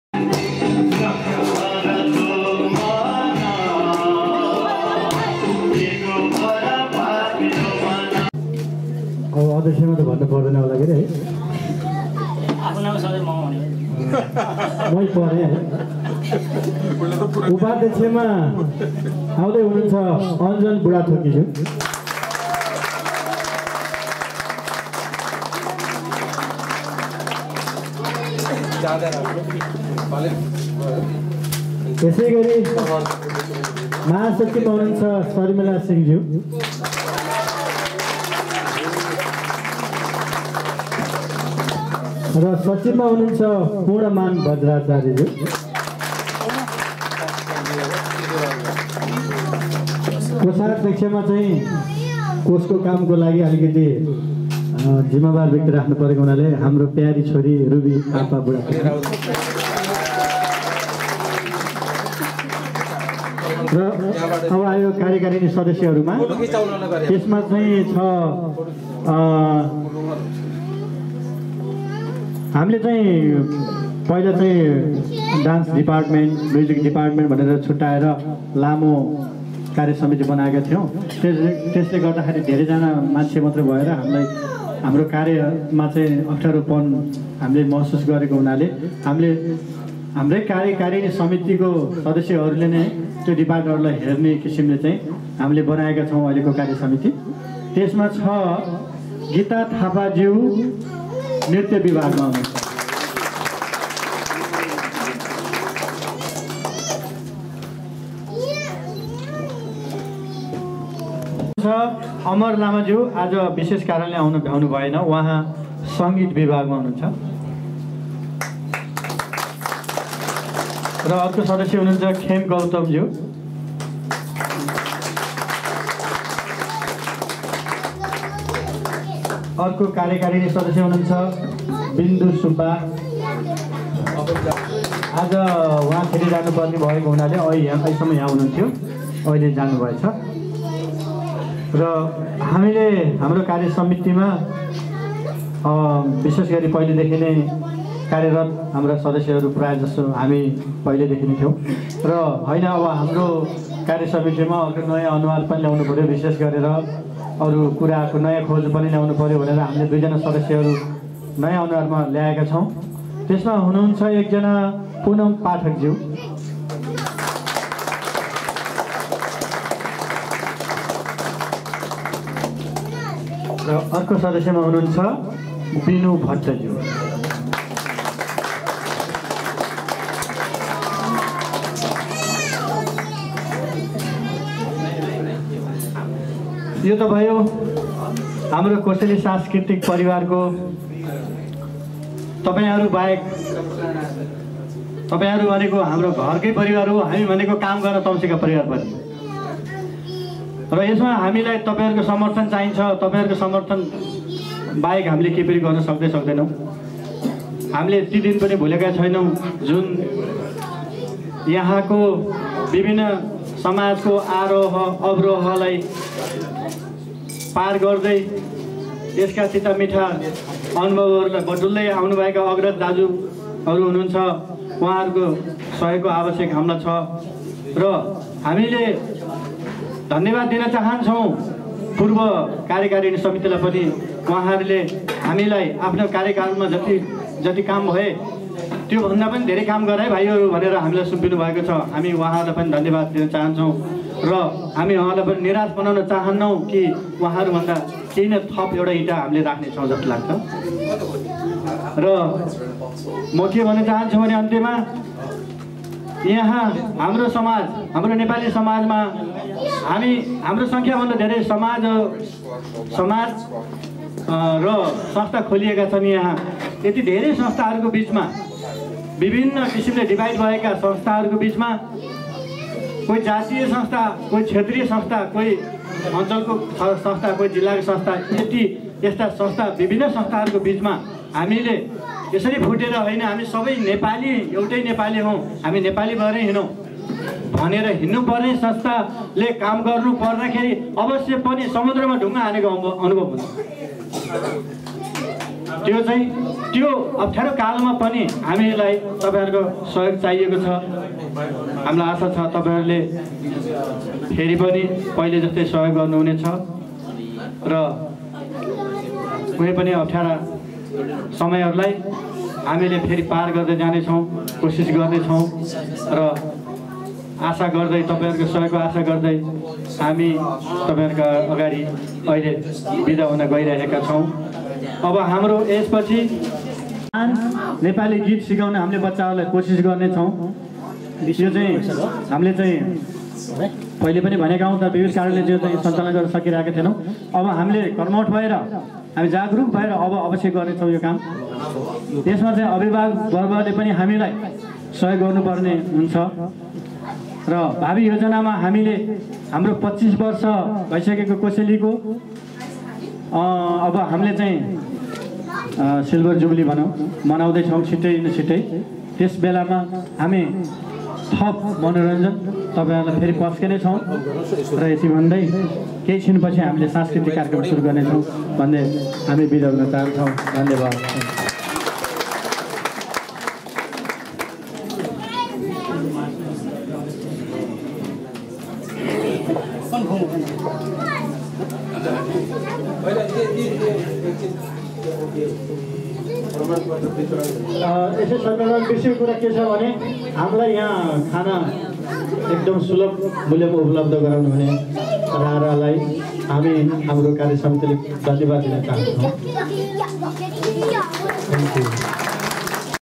Part-01: Kosheli's Sammelan & cultural program
Kosheli Nepali Cultural Fusion UK's 5th Sammelan & cultural program held in Alperton, Wembley, on 24th April 2011.